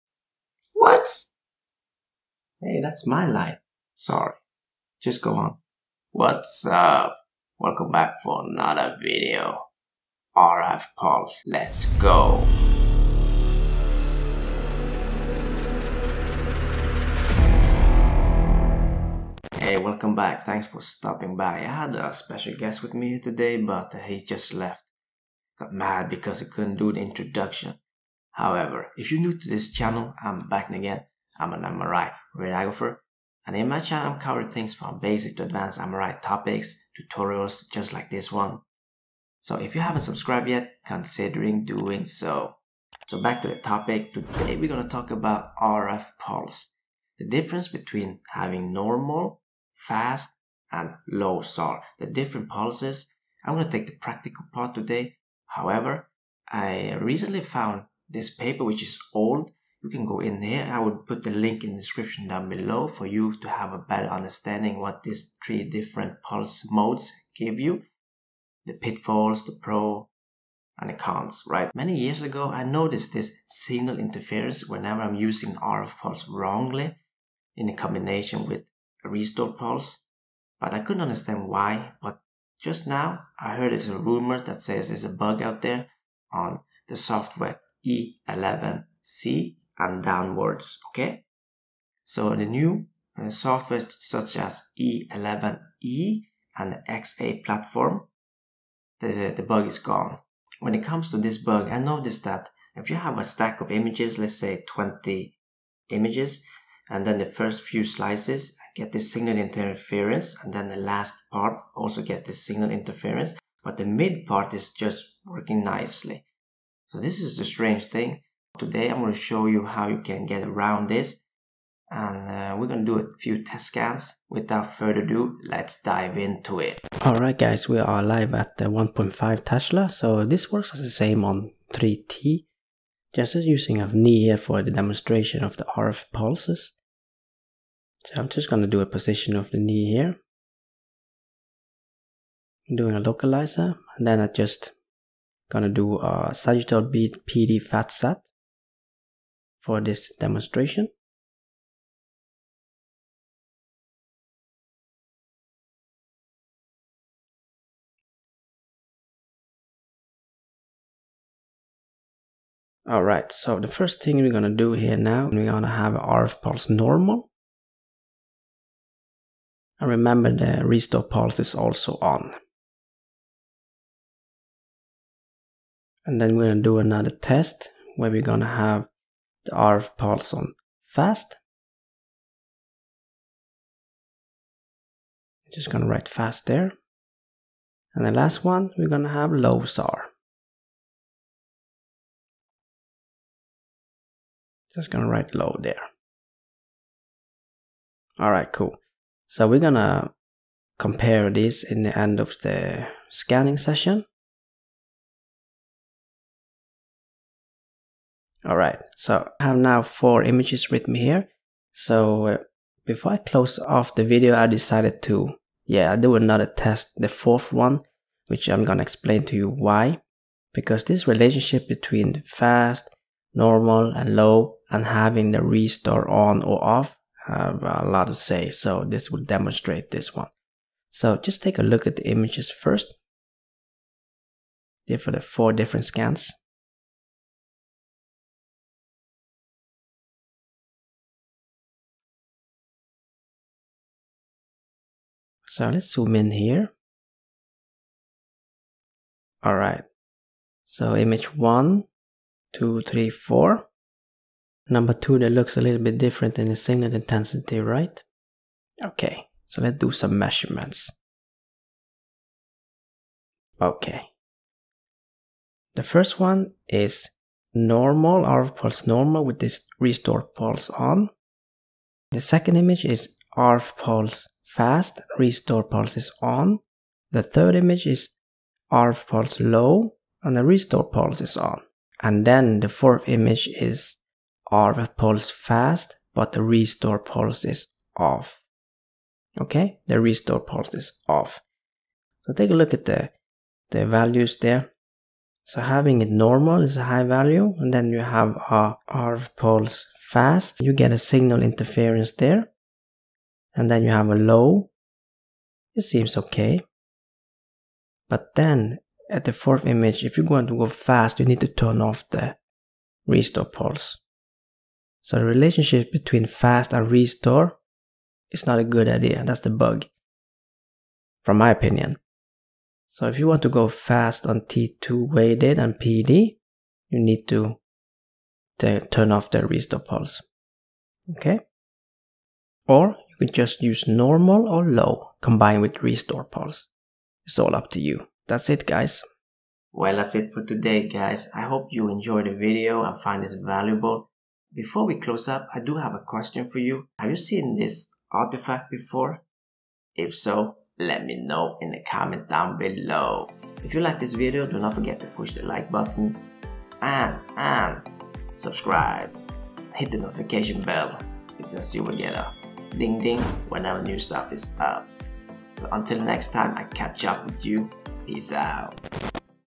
RF PULSE – A SOFTWARE BUG….
RF PULSE (FAST, NORMAL or LOW SAR). For years, I’ve notice signal/fluid interference whenever using wrong RF pulse in the combination with restore pulse. Recently, I heard it’s a bug in the software which causes this. Only on software E11c and older. On E11e and XA the bug is fixed. Nevertheless, I will demonstrate this bug for you today, and how to get around it. Join me, let’s GO….

Photo by The Nigmatic on Unsplash